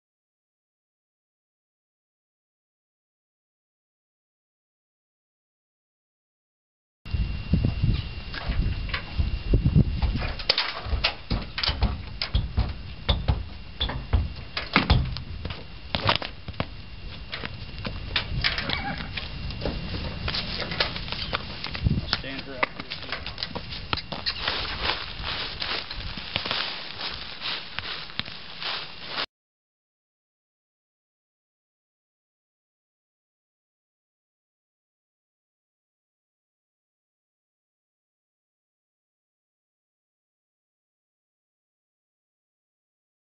Lotto Backing out of Trailer
Lotto is a 5 year old molly walking mule.  She has a sweet, compliant disposition, and a natural gait.